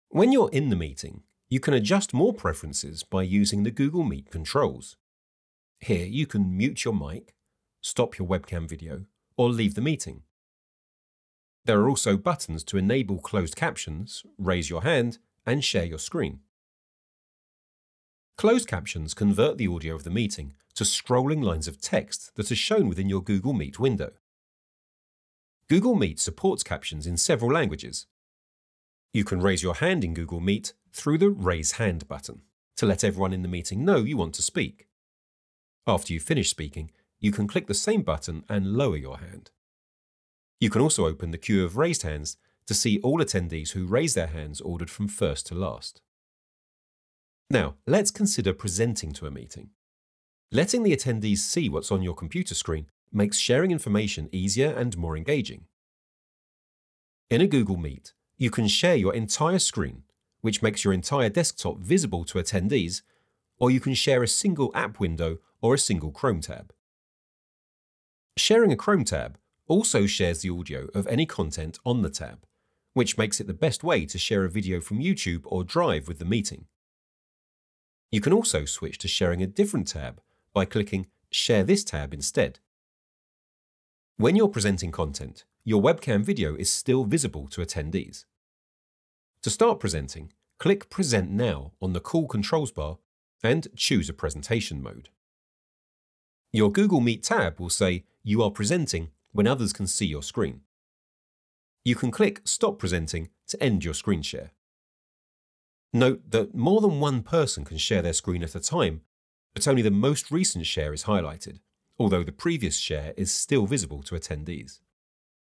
Google Meet: Use Meet controls
Google Meet is Google’s video conference software included with Google Workspace. In this video, we show you how to use Google Meet controls. To learn more about using, managing, and customizing Google Meet, check out the full training available on Google Cloud Skills Boost.